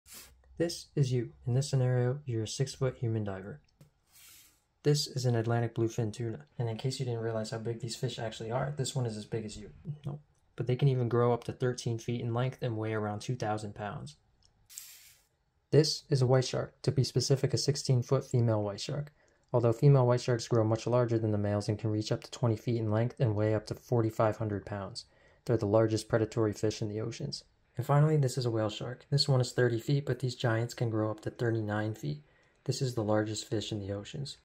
The largest Fish in the Ocean
In this short, I show you a size comparison of you and some of the largest fish in the oceans.

No, I am not a Marine Biologist.